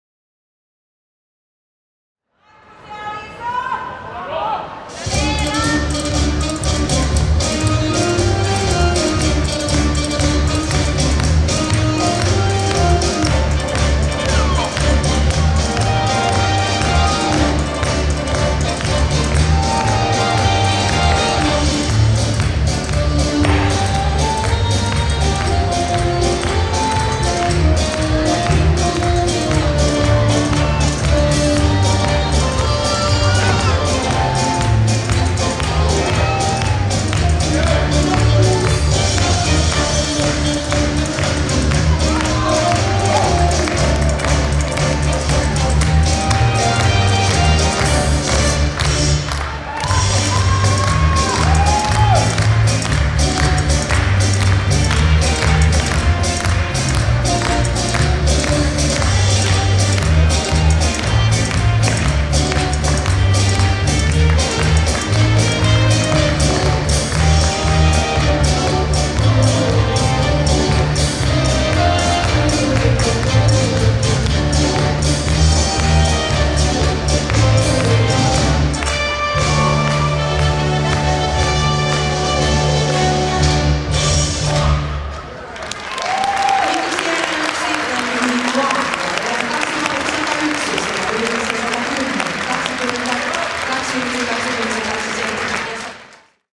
Emil Hallberg - Mona Pietilä | Pasodoble
Champion Latin
Latinalaistanssien Suomen mestaruuskilpailut
Latin SM, Swivelin 30-vuotisjuhlakilpailu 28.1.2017